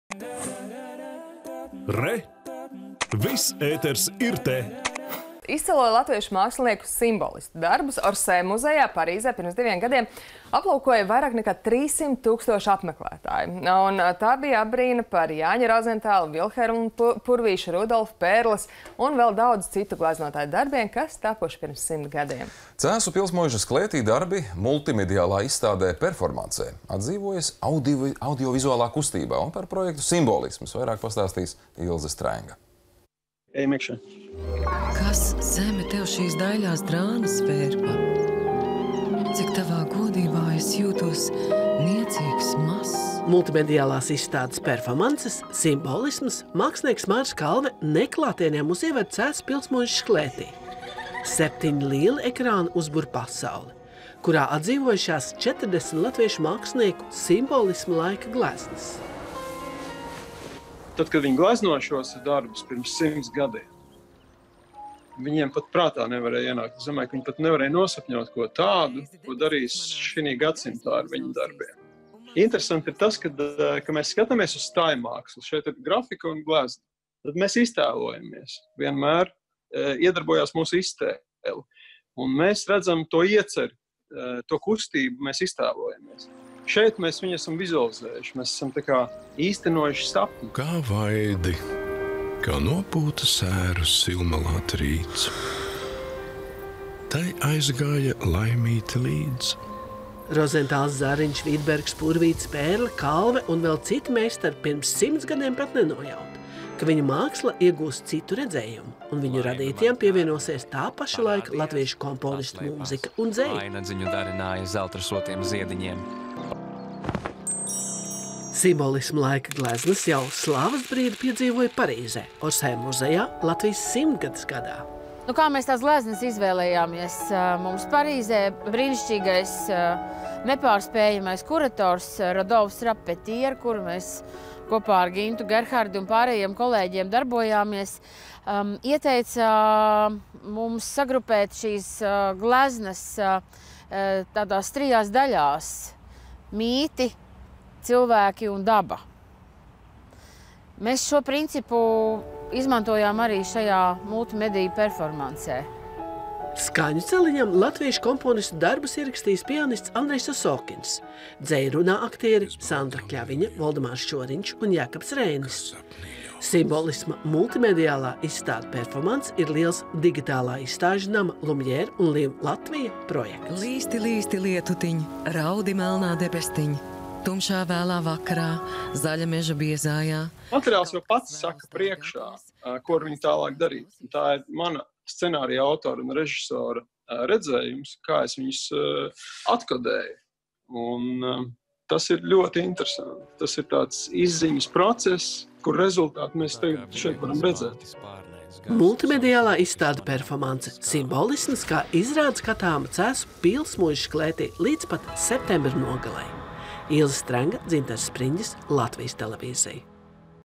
Multimediju izstāde - performance "Simbolisms" Cēsu pilsmuižas klētī
Izcilo latviešu mākslinieku-simbolistu darbus Orsē muzejā Parīzē 2018. gadā aplūkoja vairāk nekā  300 000 apmeklētāju.  Tā bija apbrīna  par Jaņa Rozentāla, Vilhelma Purvīša, Rūdolfa Pērles, Pētera Kalves un vēl citu gleznotāju  darbiem , kas tapuši pirms simt gadiem.
  Cēsu pilsmuižas klētī  darbi  multimediālā  izstādē – perfomancē atdzīvosies audiovizālā kustībā.
  Par multimediālo projektu “Simbolisms” stāsta žurnāliste Ilze Strenga.